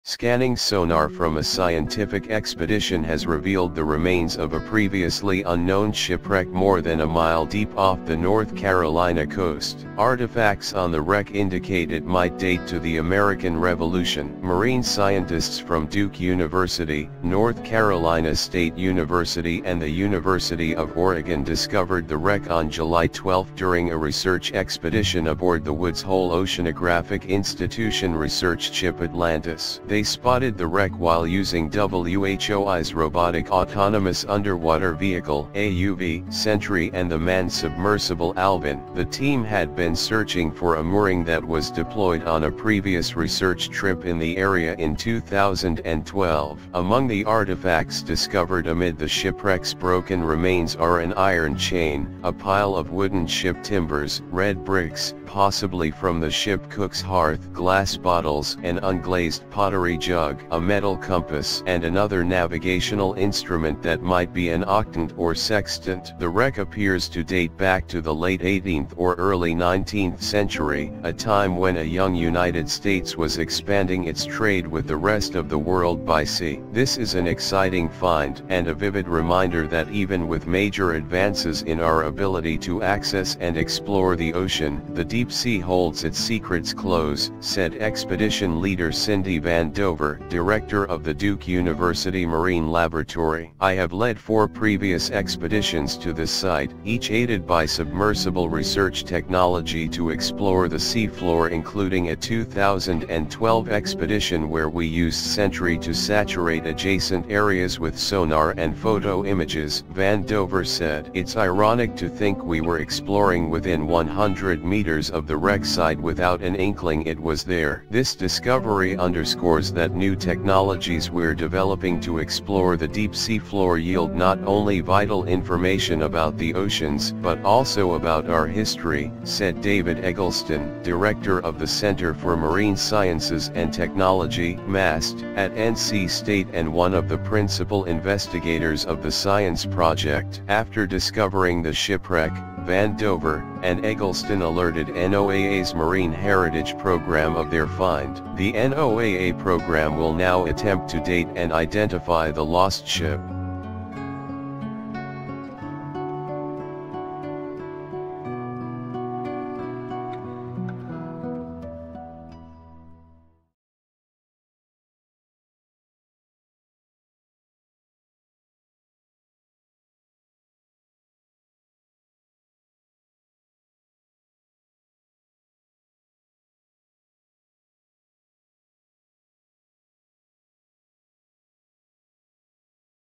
Centuries old shipwreck discovered off North Carolina coast
Scanning sonar from a scientific expedition has revealed the remains of a previously unknown shipwreck more than a mile deep off the North Carolina coast. Artifacts on the wreck indicate it might date to the American Revolution.

Marine scientists from Duke University, North Carolina State University and the University of Oregon discovered the wreck on July 12 during a research expedition aboard the Woods Hole Oceanographic Institution research ship Atlantis.

They spotted the wreck while using WHOI's robotic autonomous underwater vehicle (AUV) Sentry and the manned submersible Alvin. The team had been searching for a mooring that was deployed on a previous research trip in the area in 2012.

Among the artifacts discovered amid the shipwreck's broken remains are an iron chain, a pile of wooden ship timbers, red bricks (possibly from the ship cook's hearth), glass bottles, an unglazed pottery jug, a metal compass, and another navigational instrument that might be an octant or sextant.

The wreck appears to date back to the late 18th or early 19th century, a time when a young United States was expanding its trade with the rest of the world by sea.

"This is an exciting find, and a vivid reminder that even with major advances in our ability to access and explore the ocean, the deep sea holds its secrets close," said expedition leader Cindy Van Dover, director of the Duke University Marine Laboratory.

"I have led four previous expeditions to this site, each aided by submersible research technology to explore the sea floor -- including a 2012 expedition where we used Sentry to saturate adjacent areas with sonar and photo images," Van Dover said. "It's ironic to think we were exploring within 100 meters of the wreck site without an inkling it was there."

"This discovery underscores that new technologies we're developing to explore the deep-sea floor yield not only vital information about the oceans, but also about our history," said David Eggleston, director of the Center for Marine Sciences and Technology (CMAST) at NC State and one of the principal investigators of the science project.

After discovering the shipwreck, Van Dover and Eggleston alerted NOAA's Marine Heritage Program of their find. The NOAA program will now attempt to date and identify the lost ship.